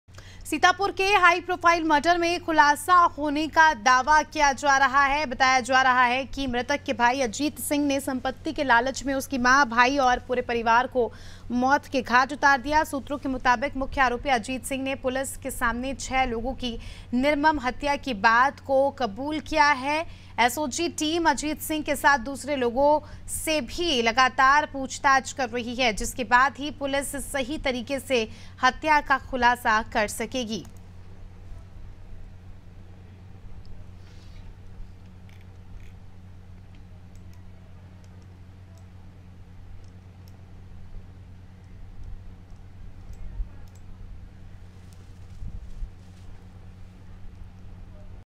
Sitapur : 6 लोगों की निर्मम हत्या का मामला, शक के घेरे में मृतक का भाई || Anaadi Tv
AIRTEL - 379